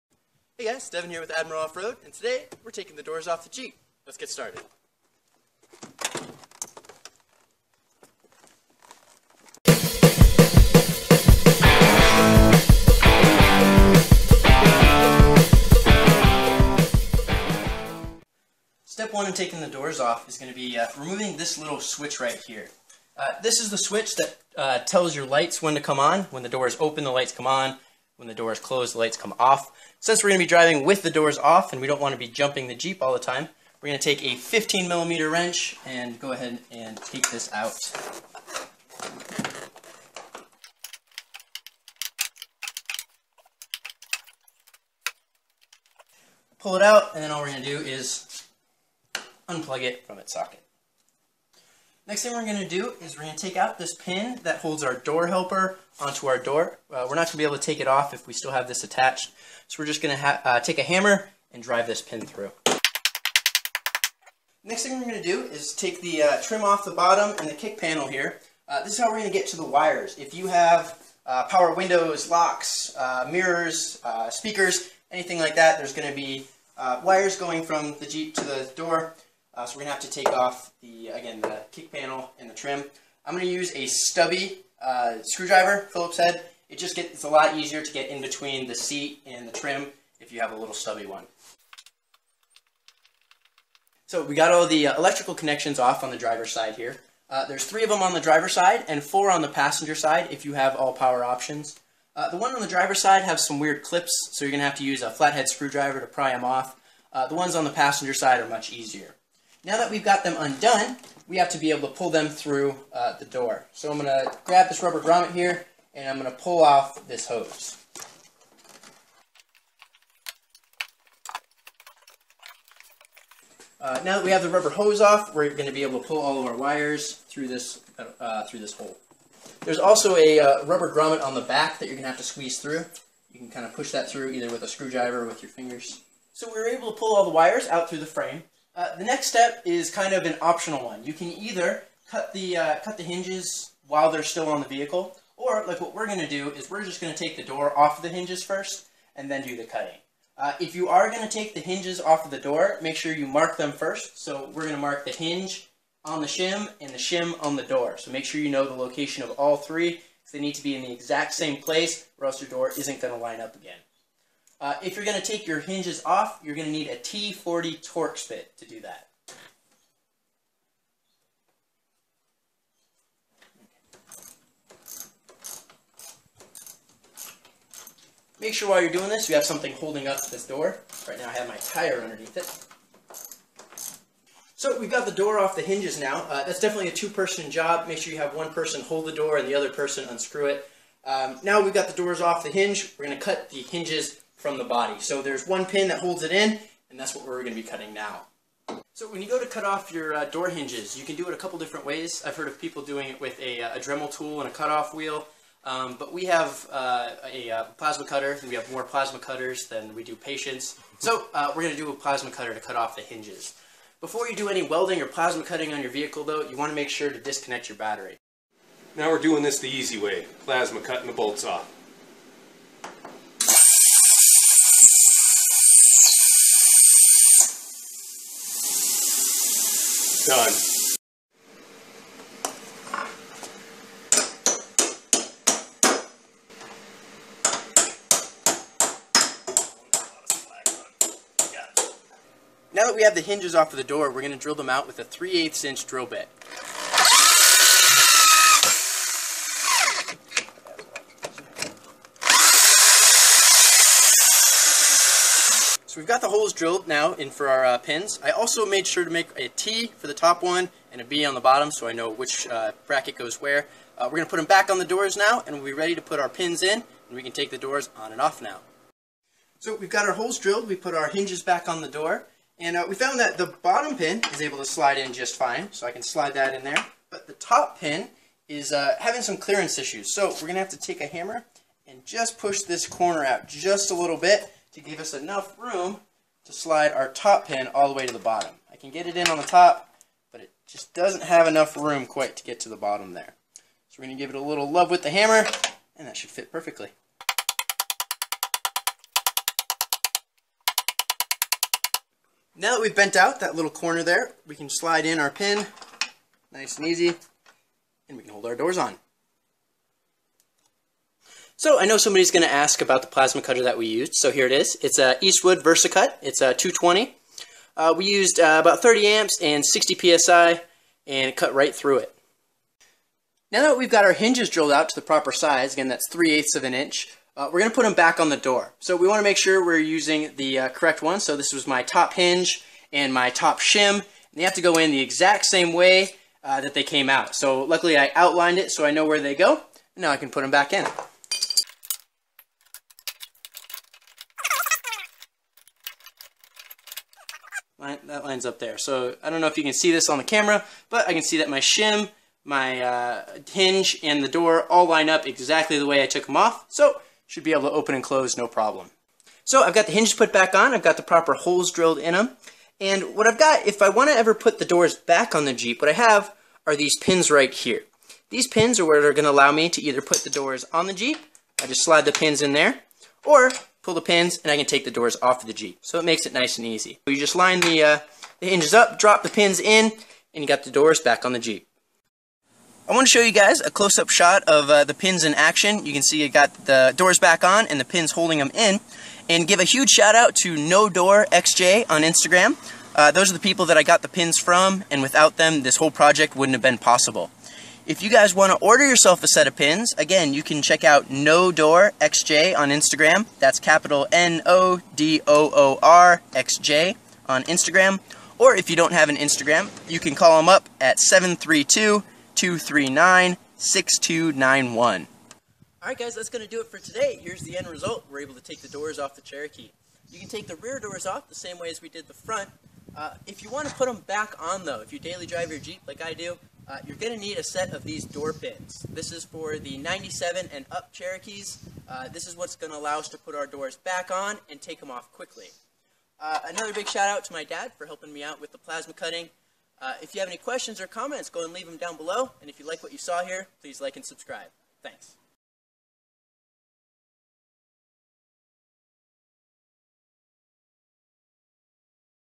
Taking The doors off a Jeep XJ (97+)
Doing the Jeep Cherokee XJ doorless mod: This is specifically for the 1997 and newer Cherokees in order to make easily removable doors.
Let him know that I sent you!